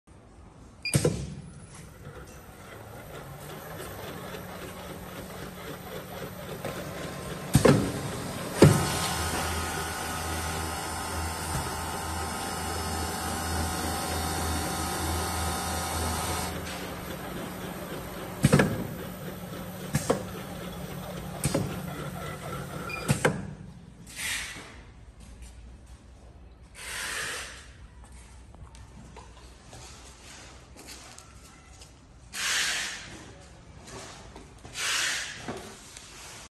Liquid filling machine
This liquid filling machine can be filled with the largest types of liquids, including highly viscous liquids. Such as a variety of pharmaceuticals, chemicals, oils, cosmetics, food, and other non-particulate liquids.